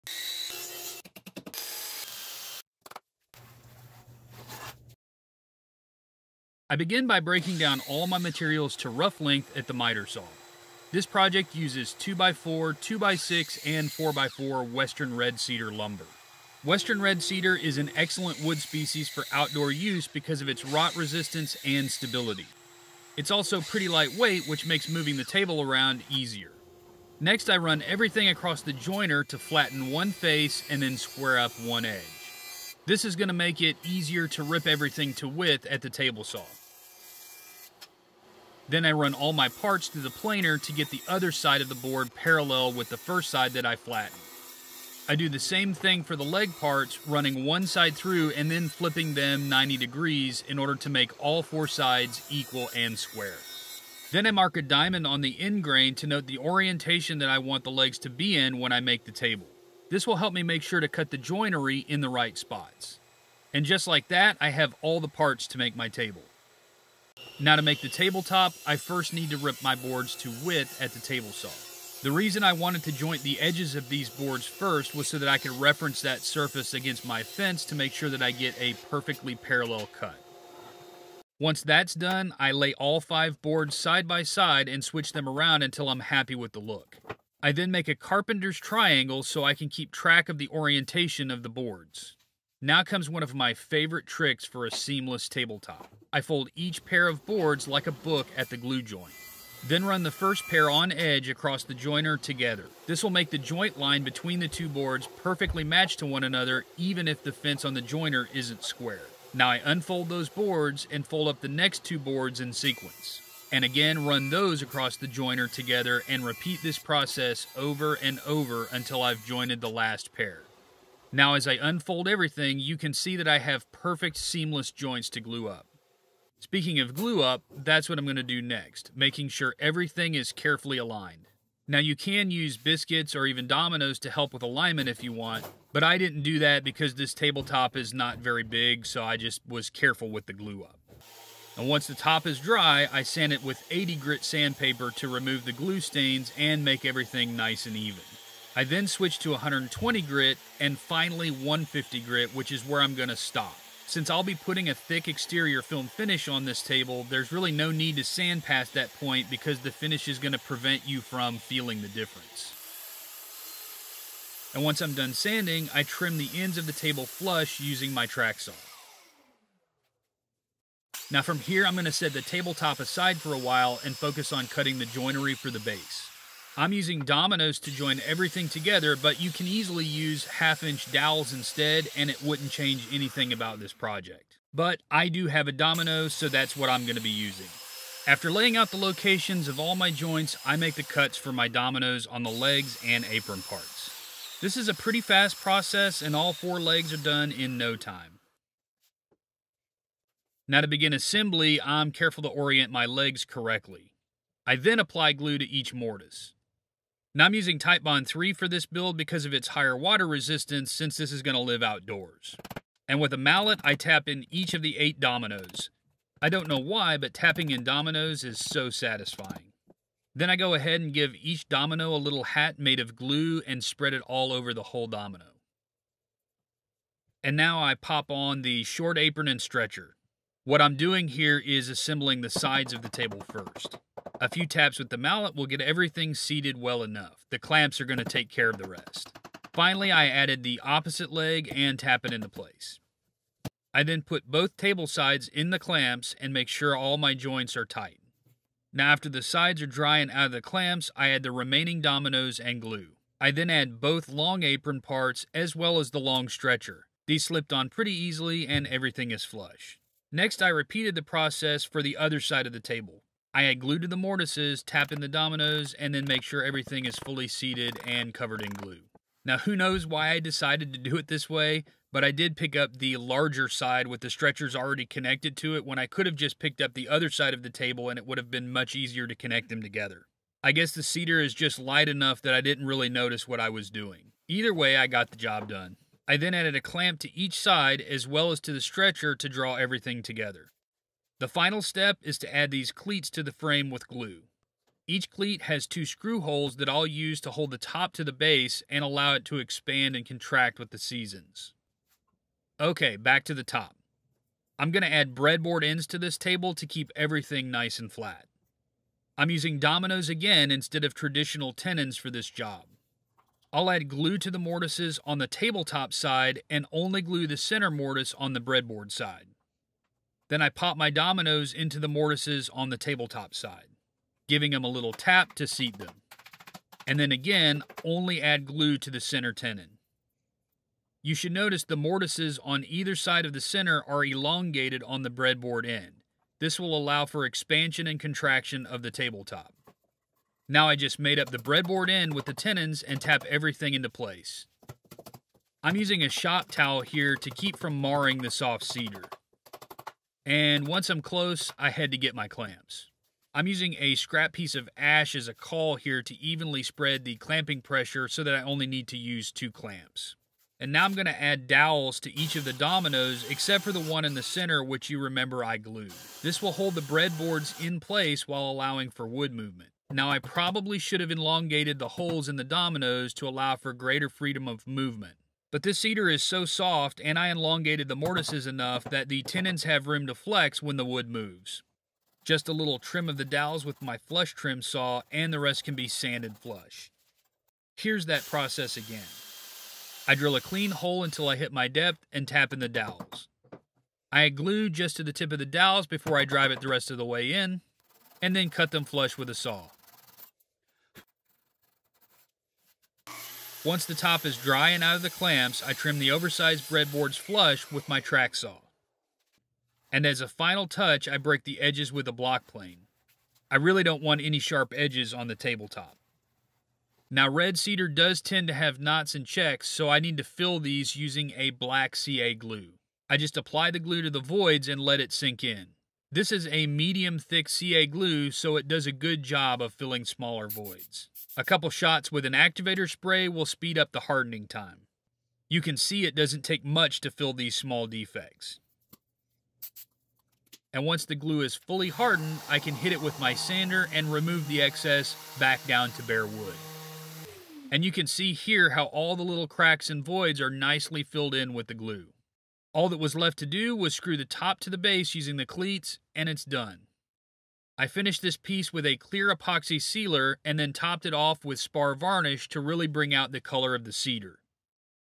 How to Build an Outdoor Coffee Table
In this video I'll show you how to build an outdoor coffee table out of western red cedar for an awesome patio addition!
►NEW MWA Woodworks Logo Merch!

LINKS TO THE TOOLS IN THIS VIDEO
►Black CA Glue
►CA Glue Activator Spray
►Water Resistant Wood Glue
►Acid Brush (for glue)
►Cheap Glue Bottle
►Parallel Clamps
►Doweling Jig (instead of a Domino)
►1/2" Dowels
►Spar Varnish
►CPES (Clear Penetrating Epoxy Sealer)

 My favorite dust mask
►RZ Mask Model M2
►RZ Mask Model M2.5